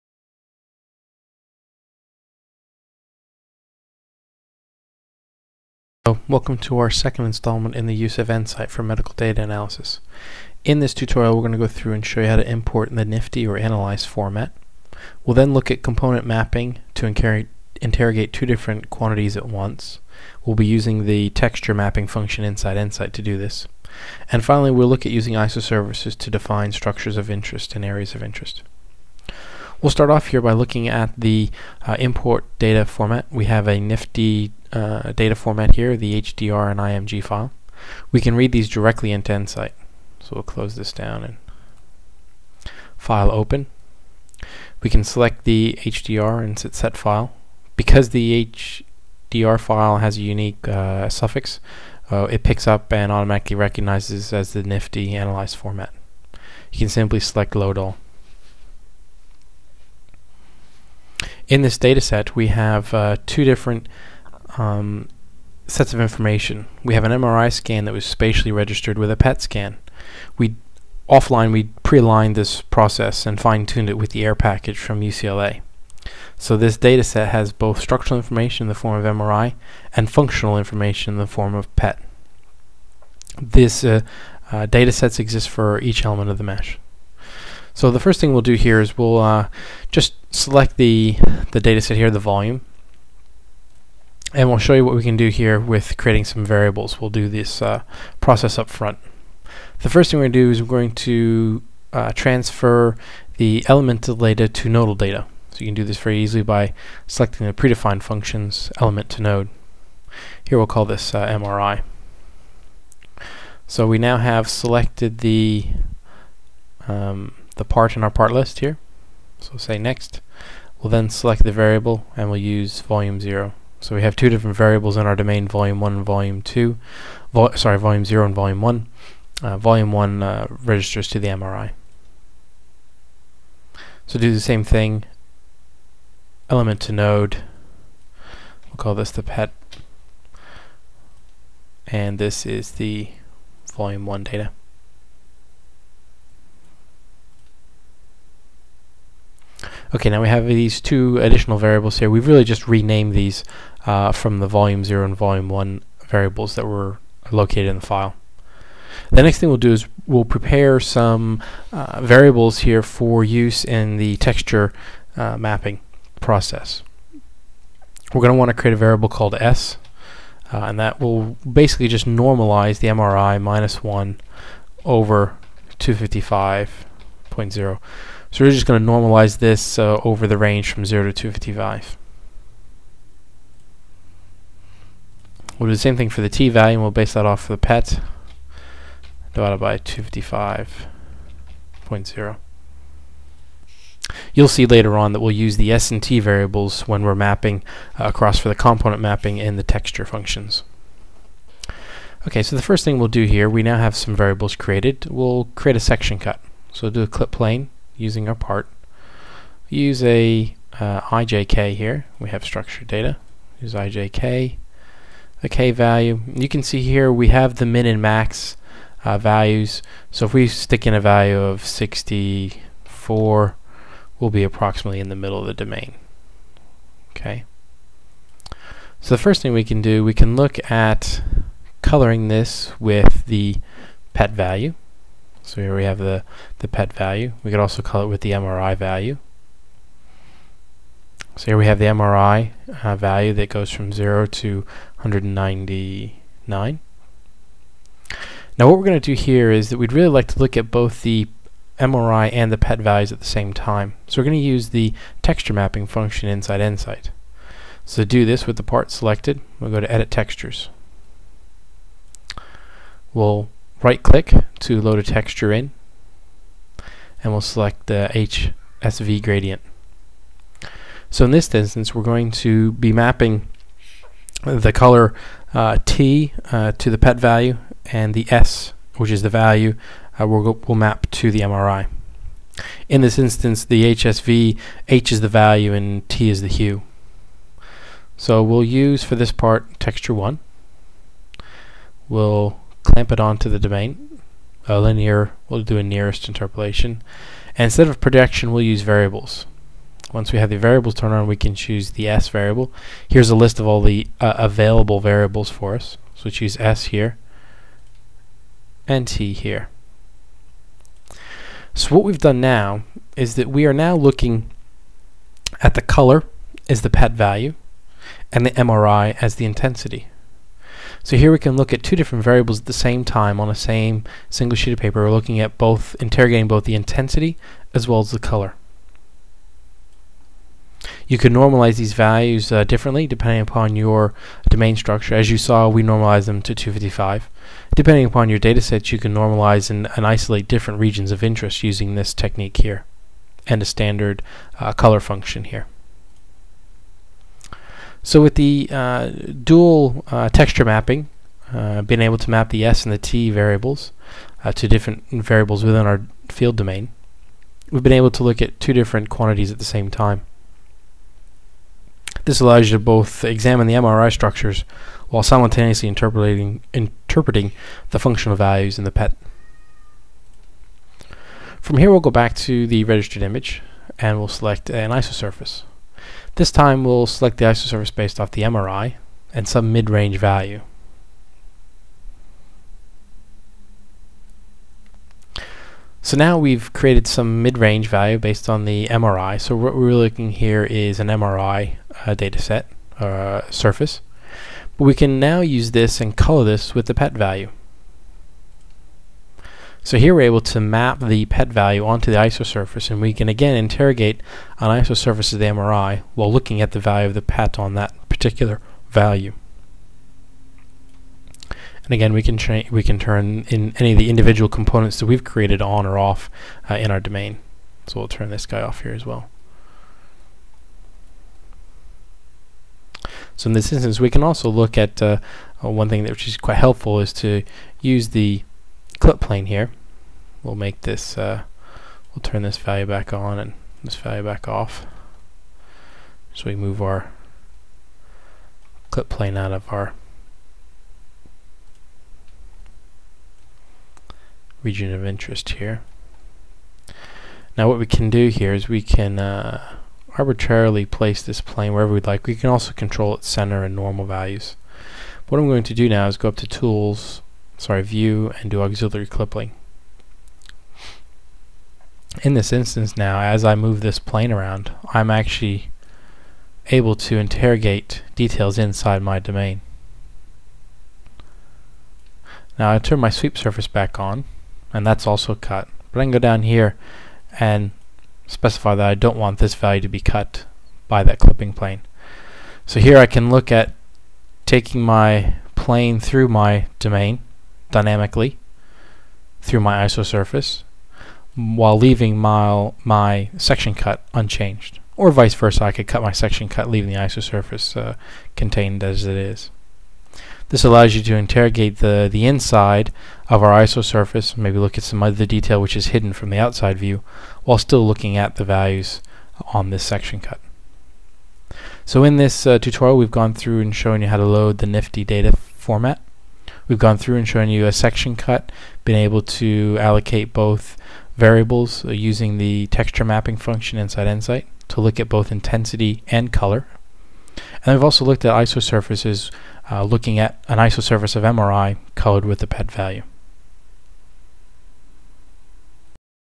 EnSight Medical Imaging Tutorial 1
Using EnSight's analysis capability, a medical dataset is imported, texture mapping used to quantify two separate fields at once, and isosurfaces used to characterize the domain